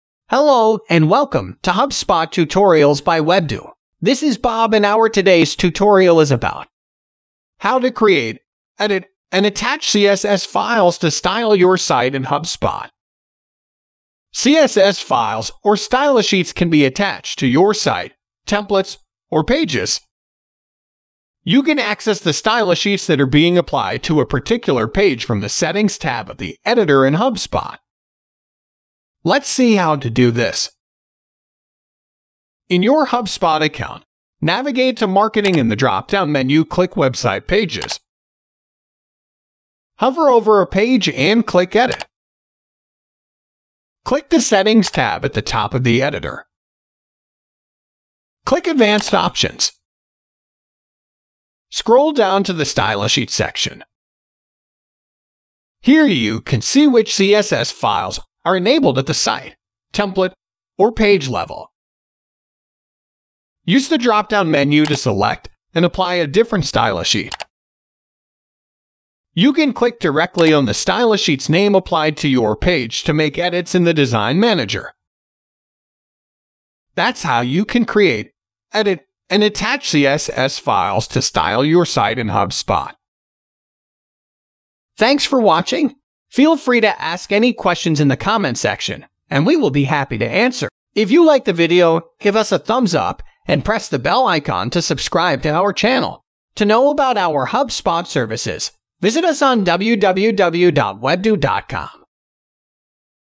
How-to create, edit, and attach CSS files to style your site in HubSpot.
In this HubSpot tutorial, we will walk you through how-to create, edit, and attach CSS files to style your site in HubSpot.

CSS files or stylesheets can be attached to your site, templates, or pages. You can access the stylesheets that are being applied to a particular page from the Settings tab of the editor in HubSpot.

Steps to follow:

1. In your HubSpot account, navigate to marketing in the dropdown menu click website pages.

2. Hover over a page and click Edit. Click the Settings tab at the top of the editor.

3. Click Advanced Options.

4. Scroll down to the Stylesheets section. Here you can see which CSS files are enabled at the site, template, or page level.

5. Use the dropdown menu to select and apply a different stylesheet.

6. You can click directly on the stylesheet's name applied to your page to make edits in the design manager.

That’s how you can create, edit, and attach CSS files to style your site in HubSpot.